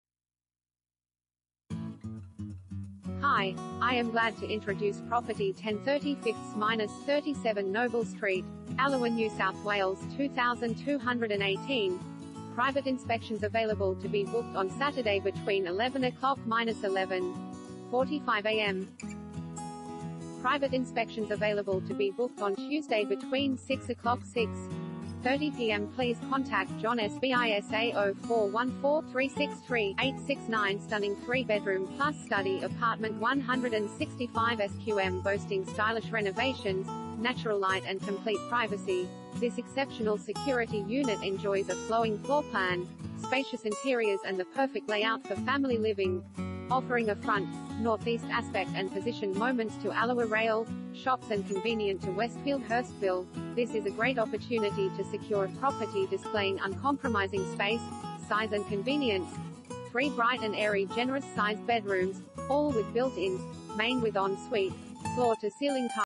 10/35-37 Noble Street, Allawah NSW 2218 | AuPropertyReport.Com
10/35-37 Noble Street, Allawah NSW 2218
Property Type: unit
Bedrooms: 3
Bathrooms: 3
Car Space: 1
PRIVATE INSPECTIONS AVAILABLE TO BE BOOKED ON SATURDAY BETWEEN 11:00-11.45AM